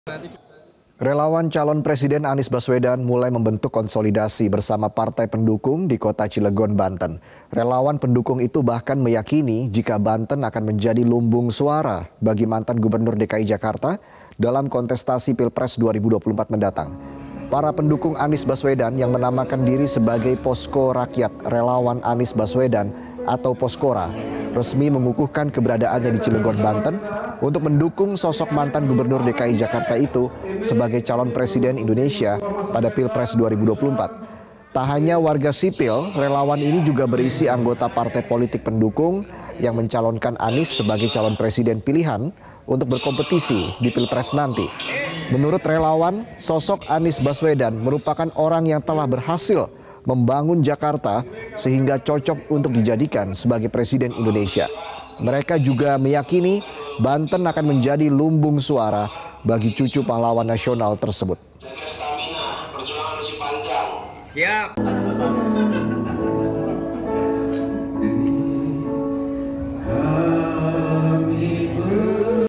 Posko Relawan Anies Baswedan Resmi Dibentuk
MetroTV, Relawan Capres Anies Baswedan mulai membentuk konsolidasi bersama partai pendukung di Cilegon Banten.

Relawan meyakini jika Banten akan menjadi lumbung suara bagi Anies Baswedan dalam kontestasi Pilpres 2024.